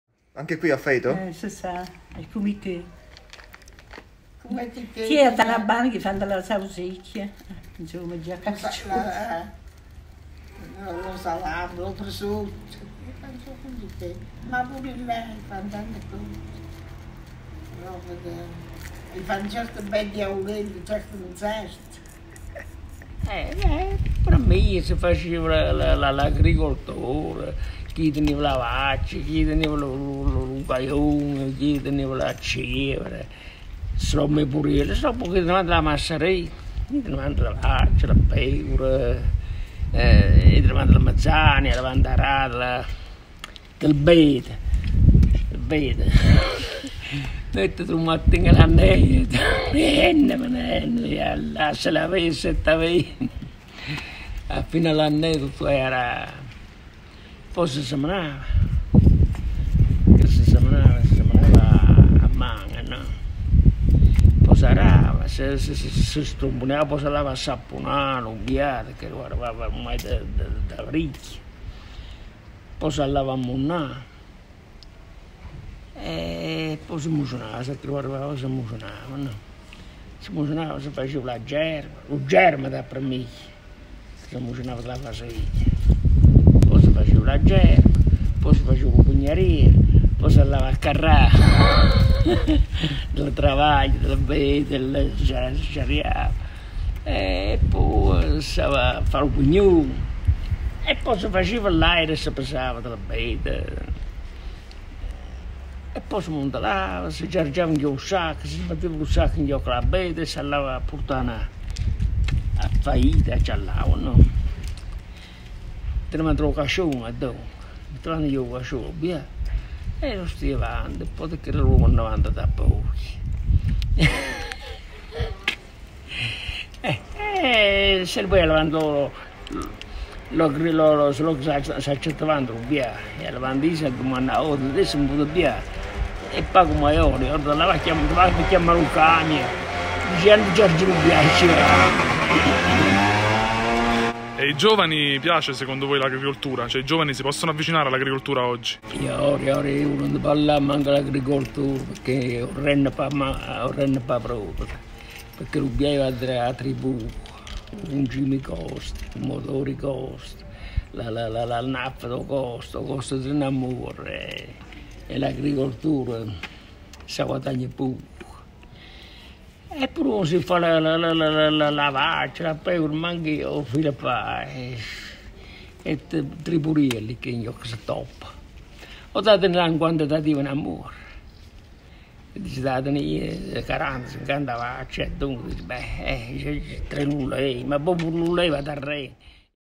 Diaologo In Franco Provenzale
Oggi è un giorno di ricorrenza e festeggiare l’inizio di un cammino a passi lenti è doveroso, auguri Francigena Walking Tour.
Il primo settembre 2019, a distanza di un anno, iniziammo un viaggio insieme a Luca Vernacchio e Simone Ielardi nelle le voci dei paesi d’Irpinia, da Casalbore a Greci attraverso la via Francigena. Oggi per celebrarlo vi lasciamo una testimonianza in Franco Provenzale, di un giovane contadino che ha in testa la terra e nel cuore il paese.
Ieri siamo stati a Faeto con l’amico Luca, siamo andati dopo Greci, dove ci sembrava finisse la terra.
Vincenzo Girardi
radicato a Faeto da novant’anni.